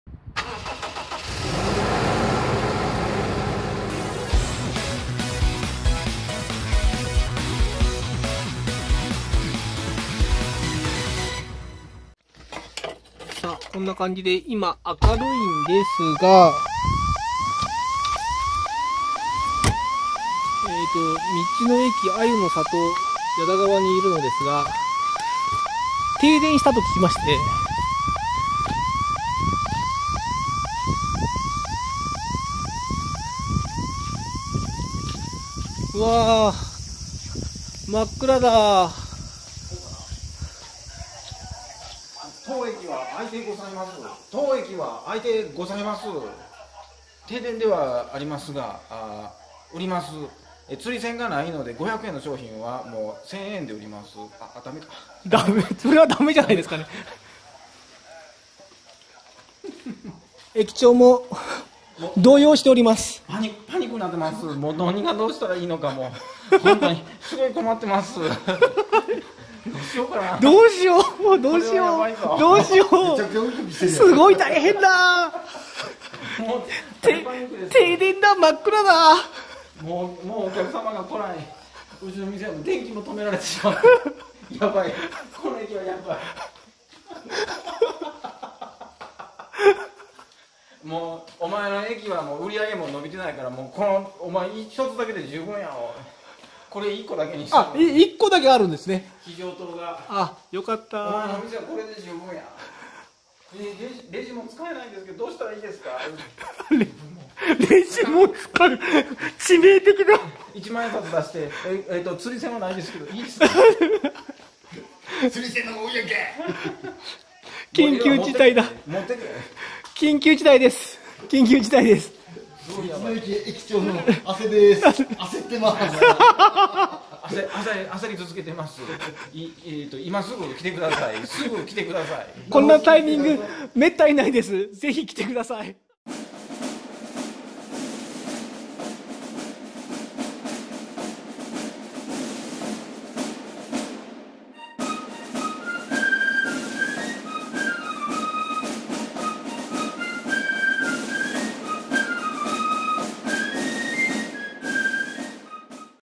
【キャンピングカー】道の駅あゆの里矢田川・大停電【日本一周中夫婦】
道の駅あゆの里矢田川 が停電になってしまう危機が発生！！
道の駅はどうなってしまうのか！
そして駅長は？！？

キャンピングカーで日本一周 をしているがっちゃんとむっちゃんです。
家を断捨離して、毎日キャンカーの中で車中飯と車中泊の生活をしています。
ドキドキワクワク波瀾万丈になっていくであろう旅行記です。

今回は【道の駅あゆの里矢田川での停電トラブル 】を紹介します。

◆ブログでは旅先だけでなく観光地や道の駅の情報も書いています☆

☆機材提供　渡邉カズヨシ@bcj70ut2hSBV0iF